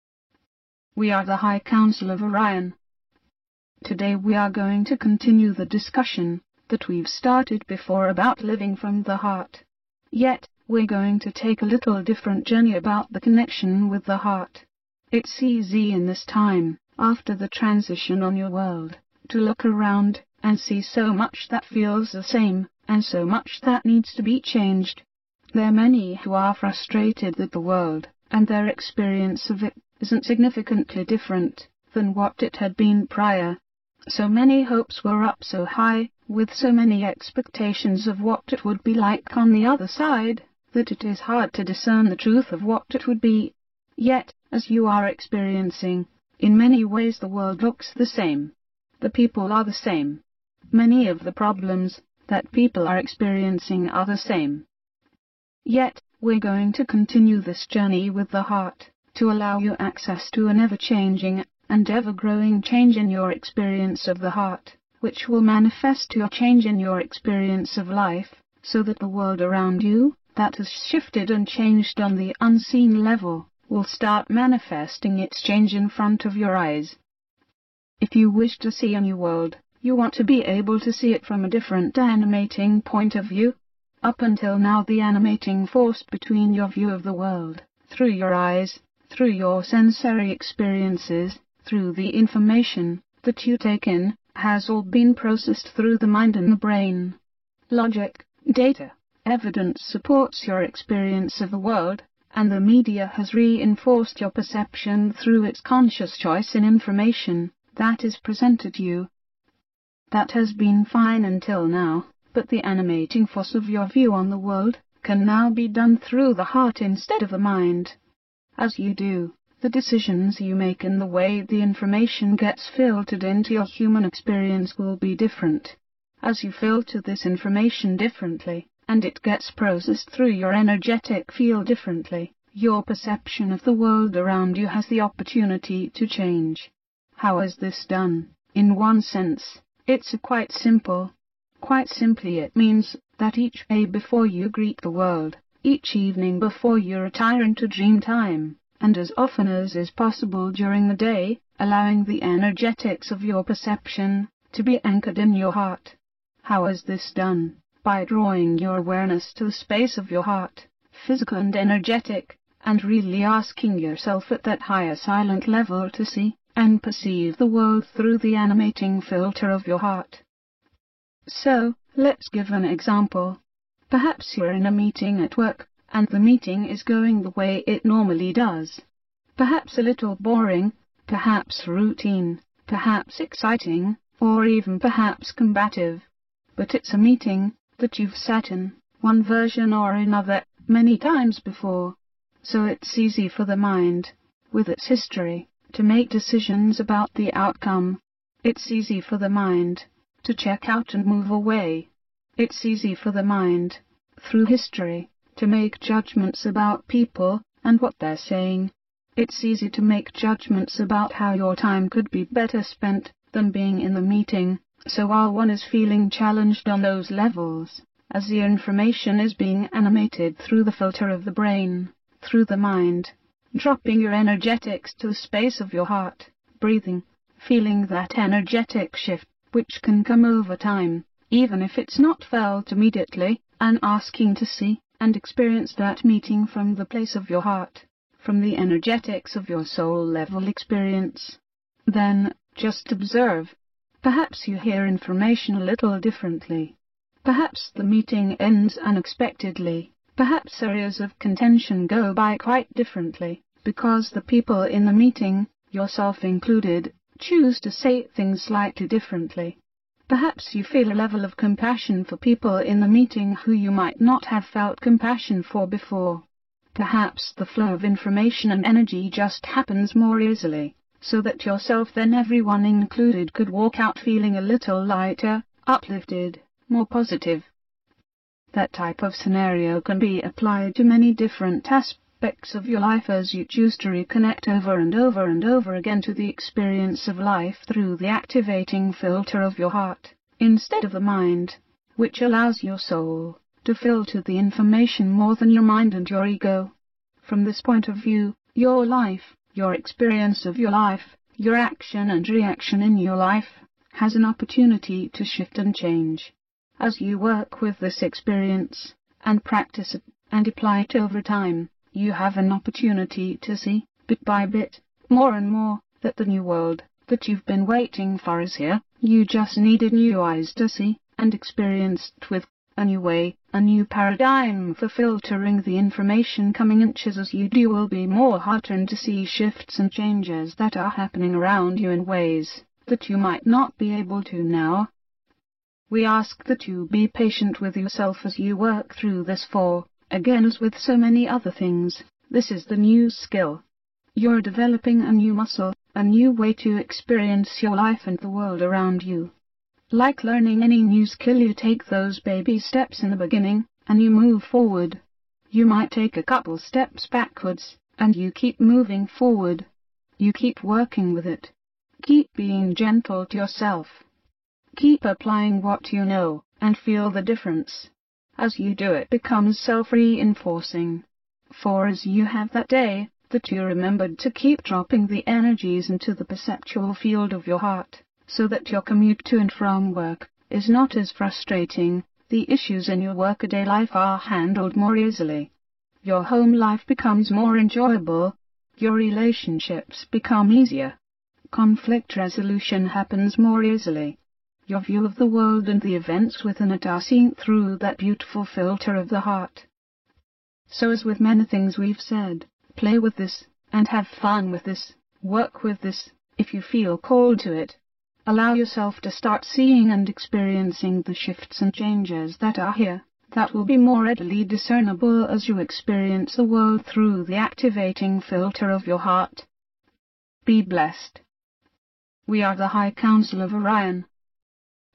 High Council of Orion February 8, 2013
High Council of Orion Star System weekly message

Welcome to the New World. Change is here and it's here to stay. Accept it don't be afraid of it.

It could quite be the best thing that will happen in human history.

Images from NASA HST of Orion Belt'